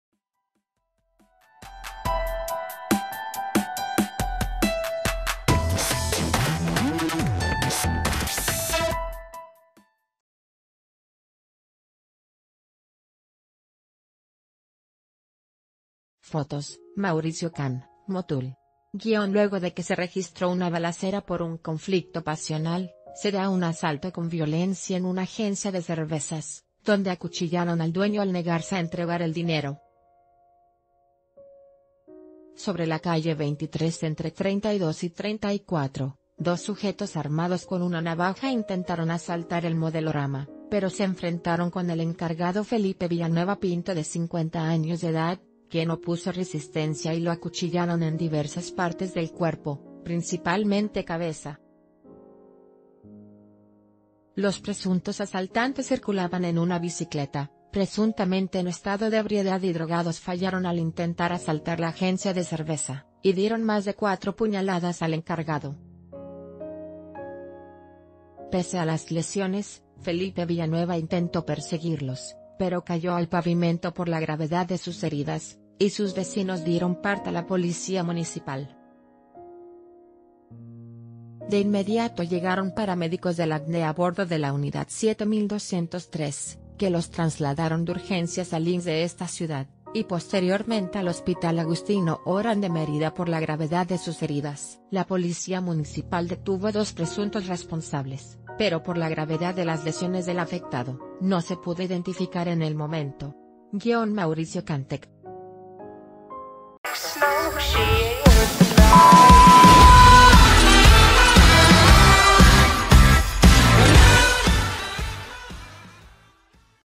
Apuñalan a encargado de una tienda en Motul - El Diario de Yucatán
(Fotos: Mauricio Can) MOTUL.- Luego de que se registró una balacera por un conflicto pasional, se da un asalto con violencia en una agencia de cervezas, donde acuchillaron al dueño al negarse a entregar el dinero. Sobre la calle 23 entre 32 y 34, dos sujetos armados con una navaja intentaron asaltar...